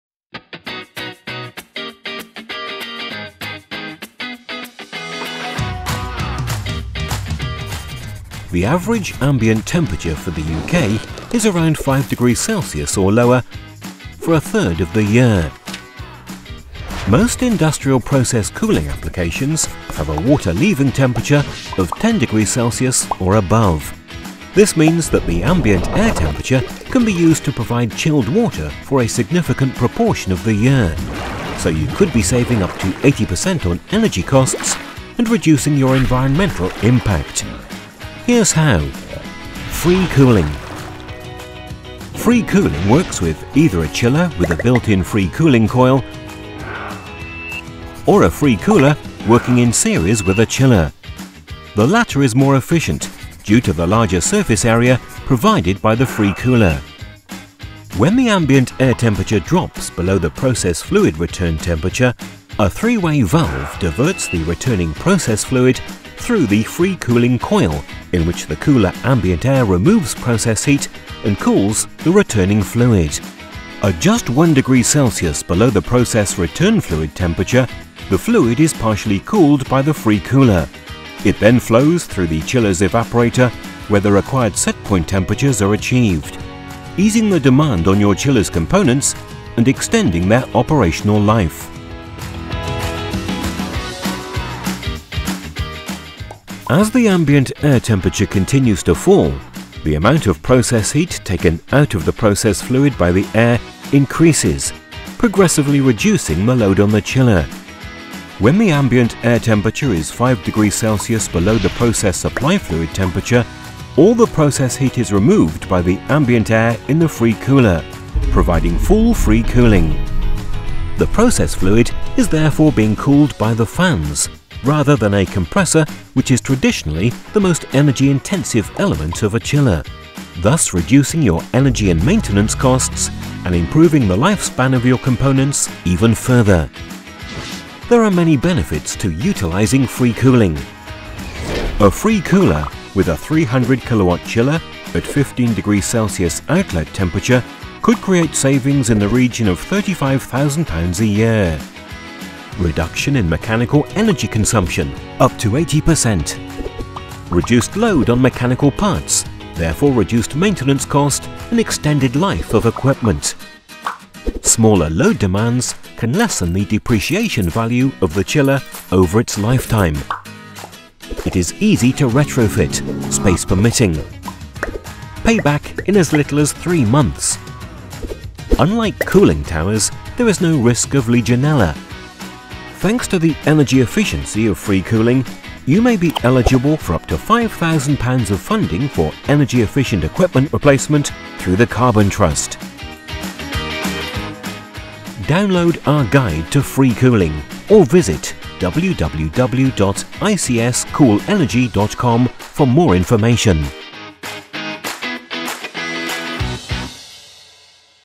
Free Cooling - How Does It Work?
How Does Free Cooling Work? Watch our short video to find out how you can use Free Cooling to reduce your cooling energy costs by up to 80% and improve your environmental profile.